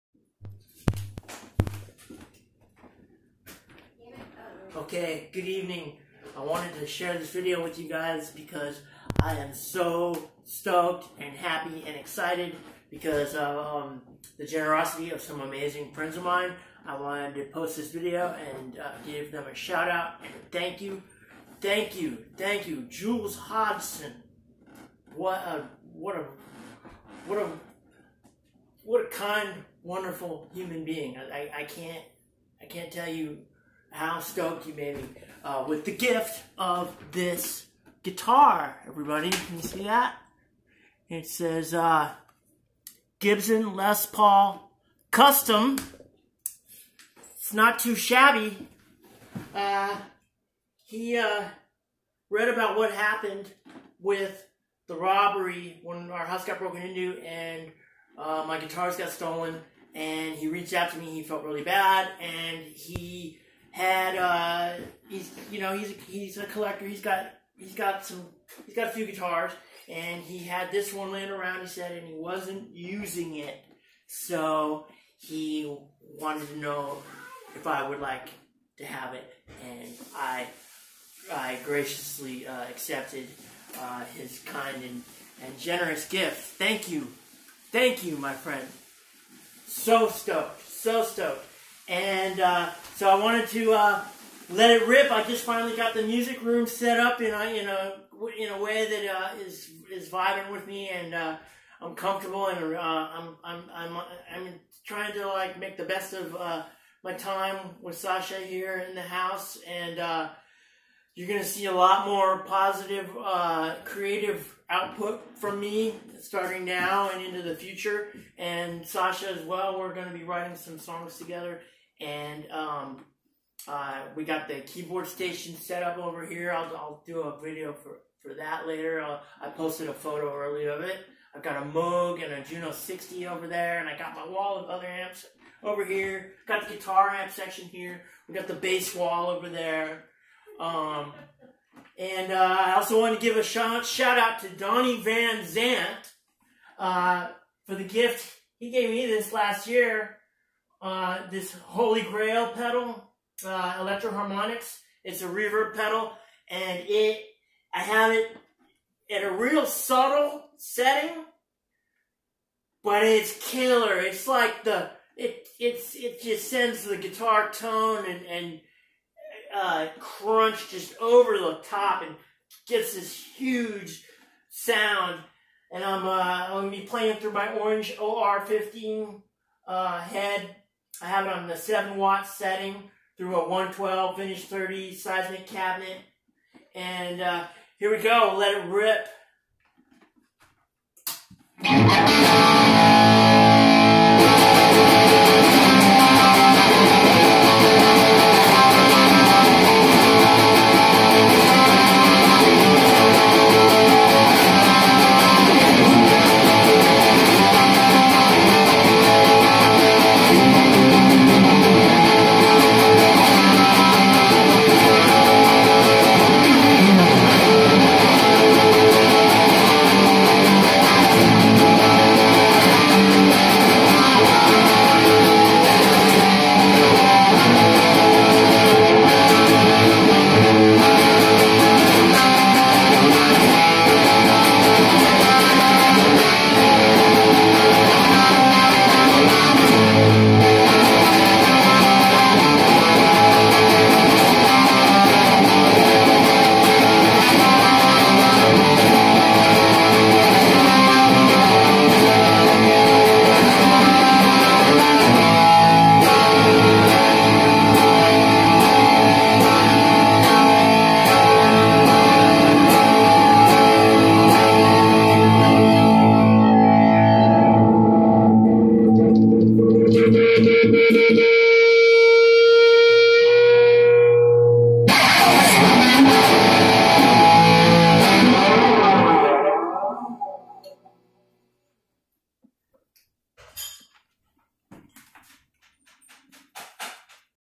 Thank you Jules Hodgson for the Les Paul
Thank you for the guitar, Jules Hodgson! Jules reached out to me when he heard about our house getting robbed and my guitars stolen and wanted to help me out with the gift of this fine guitar. He shipped it to me right away from Seattle!! Also shout out and thank you to Donny Van Zandt for the EHX Super Reverb pedal! I made this riff up on the spot for this video.